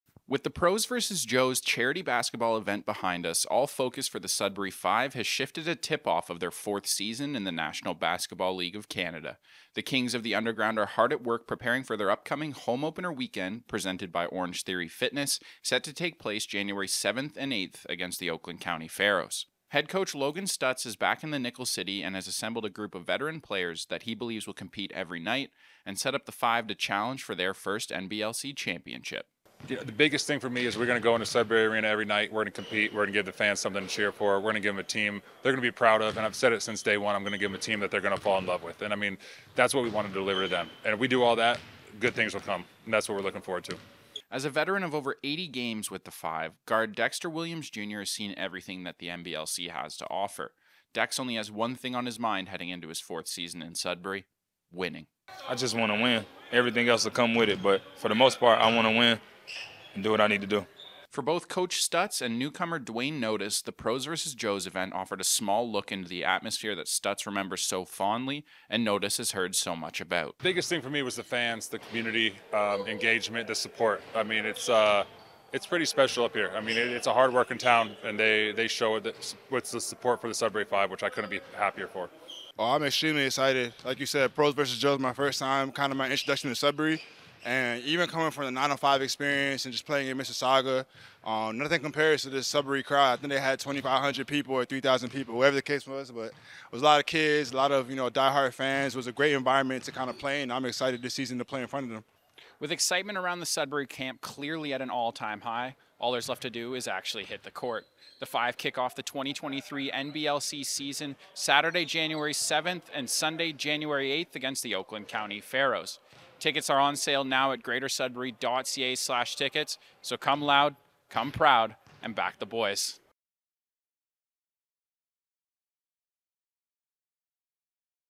Sudbury FIVE 2023 Season Preview
The Sudbury FIVE are almost set for tip off of the 2023 season.

Head to greatersudbury.ca/tickets and back the boys!